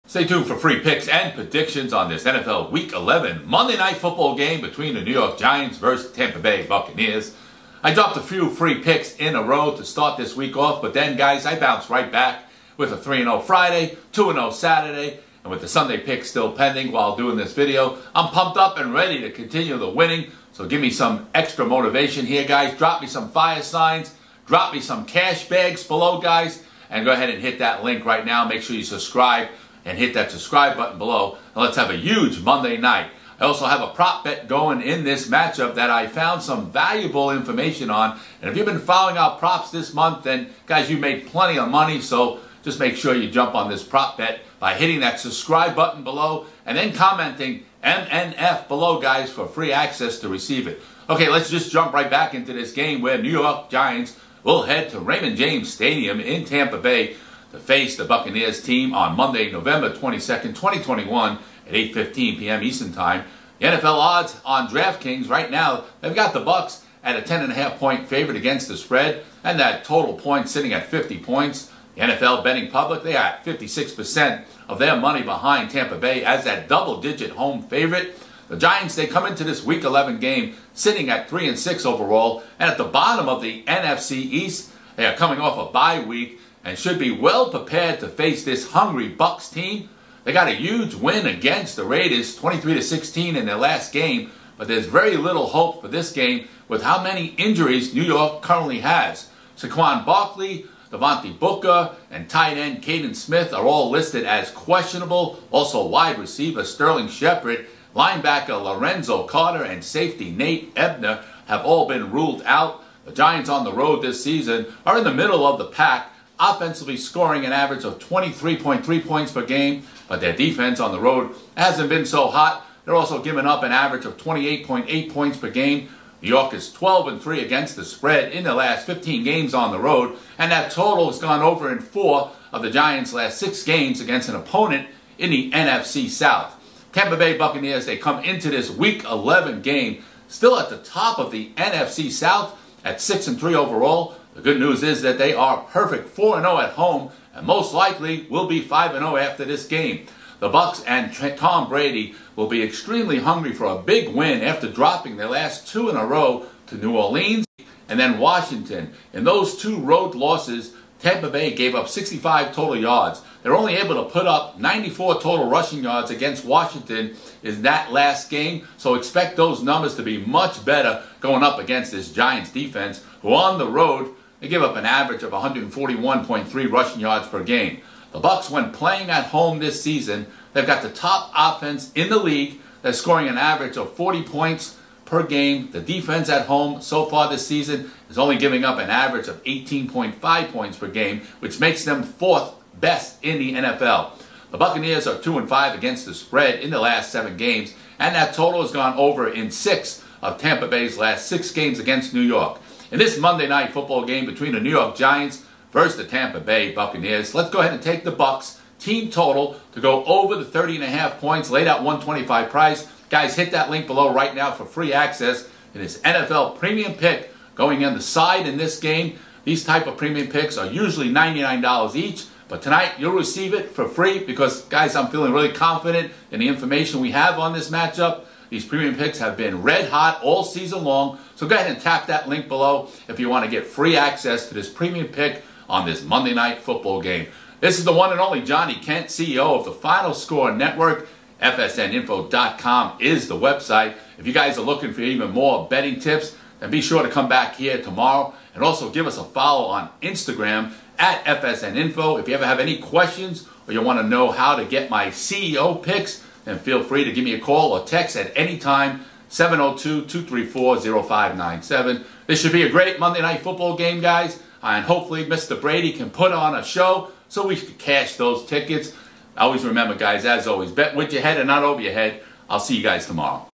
NFL Picks - New York Giants vs Tampa Bay Buccaneers - Monday Night Football
Giants vs Buccaneers Prediction and NFL Picks Today on this NFL Week 11 Monday Night Football game.

Plus Free Picks on the side in this game tonight by going

The Buccaneers vs Giants kicks off this Monday Night, November 21, 2021 at Raymond James Stadium in Tampa, FL. The DraftKings NFL Odds currently have the Bucs listed as a -10.5 points favorite against the spread and the over/under at 50 total points. Tom Brady and the Buccaneers are coming off back to back road loses  where they're defense played horrible, so this is the perfectly opportunity to get a bounce back win against the struggling and injured New York Giants.